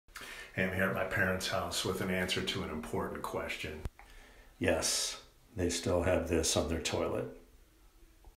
Wilcox's Parents Have A Special Toilet Seat
Wilcox heads to his parent's house and finds something a little special in the bathroom.  Who else has something like this in their bathroom?  What's the oddest thing you've found in someone else's bathroom?